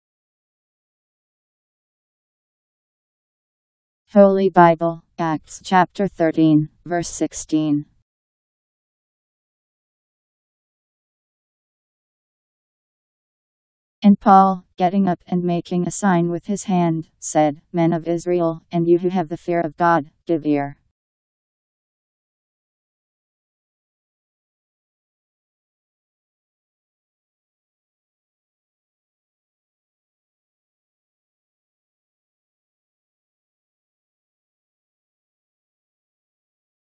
Holy Bible Acts 13:16
Video shows Holy Bible Acts Chapter 13 Verse 16: And Paul, getting up and making a sign with his hand, said, Men of Israel, and you who have the fear of God, give ear. Powered by MaryTTS. Text taken from Bible in Basic Edition. Bible Acts 13:16 Bible Learning, what Bible verse says.